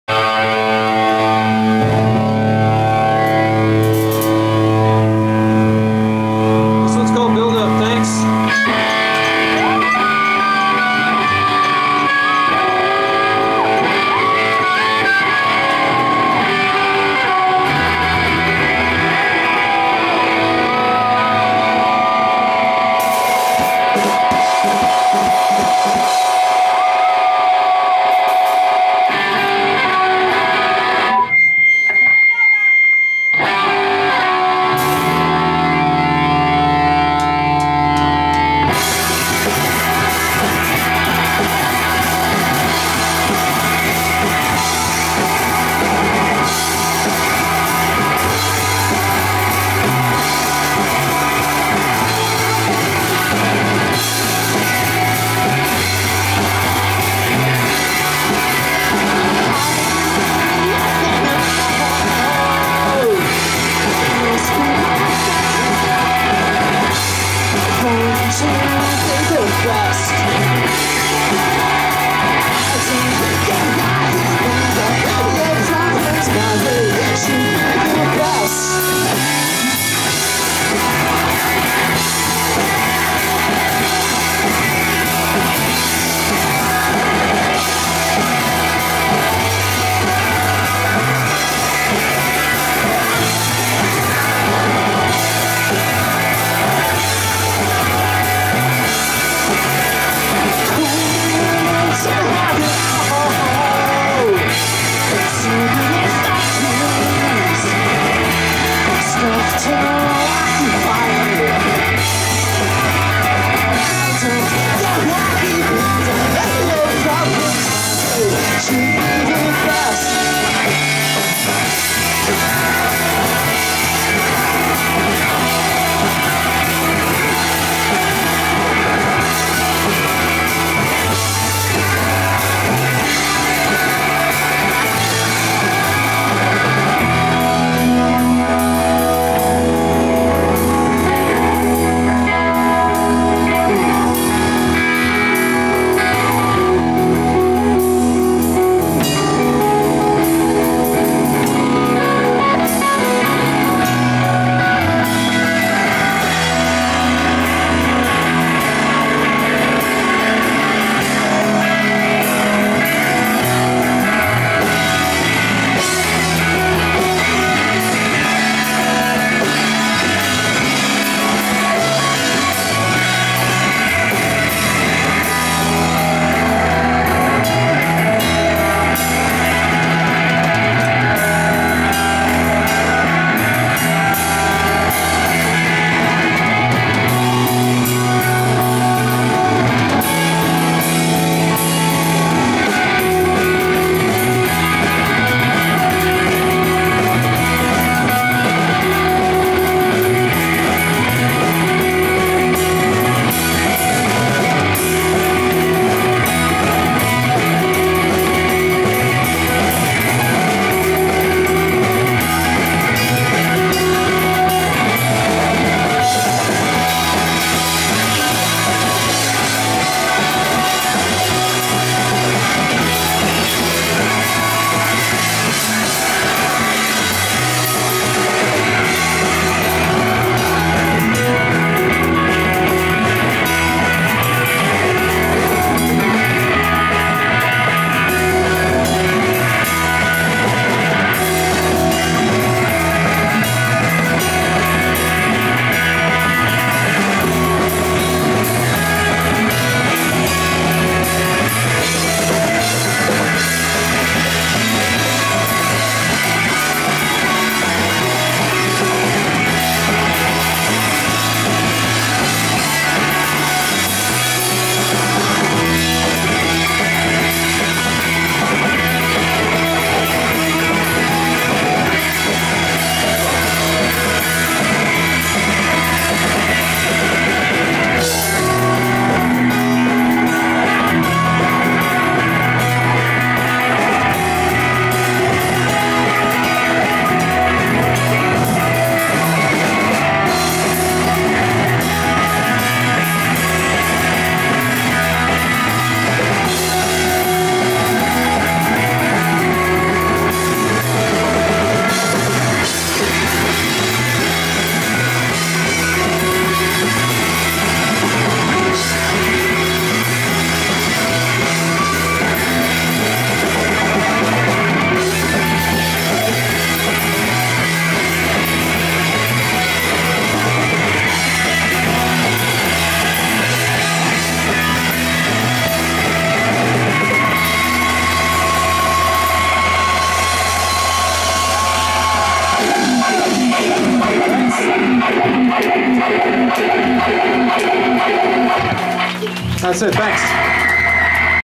Et Al @ Ottobar [9/9]
Baltimore math punks Et Al upstairs at Baltimore's Ottobar.